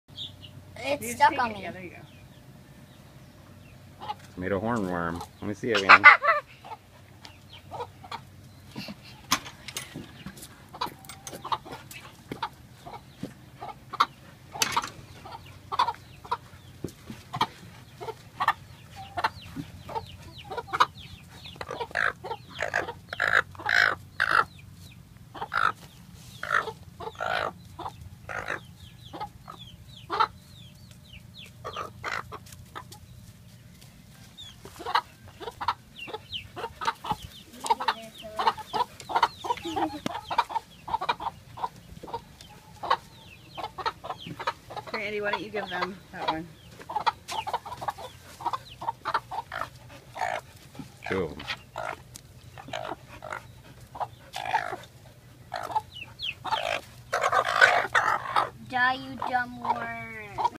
Doomsday for the Tomato Hornworm
Caterpillar lovers beware!  My chickens are hungry!

We found these devouring our tomato plants